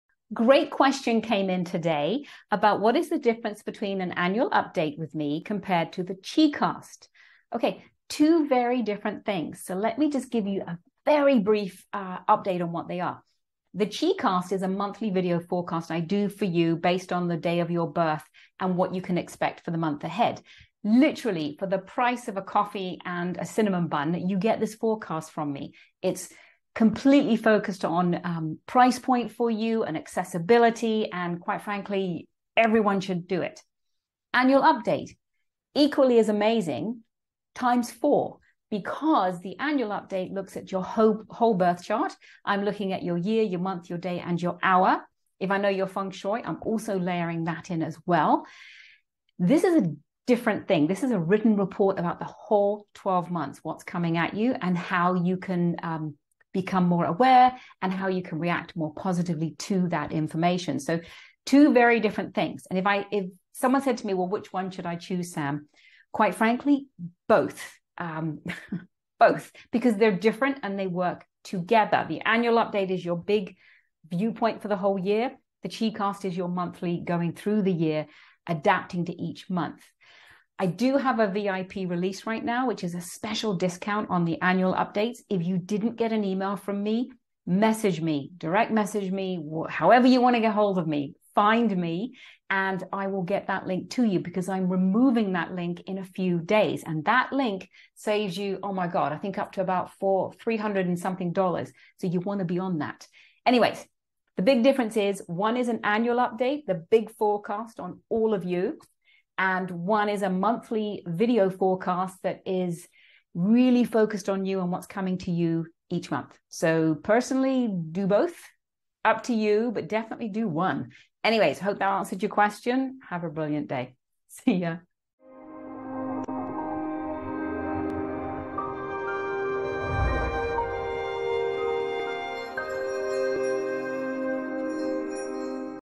Which Type of Forecasting Is Better For Me?
In an economy that is strikingly unbalanced, chaotic and fearful... you need to find your foundation and get a firm footing.

Had a super interesting question about the two types of Astrology Forecasting I provide; which one is better, and why...?

Well, best thing I can suggest is you take a listen to my description of both of them and you'll see why you need BOTH!

Once you have A Sam, you'll wonder how you ever got along without one 💁‍♀️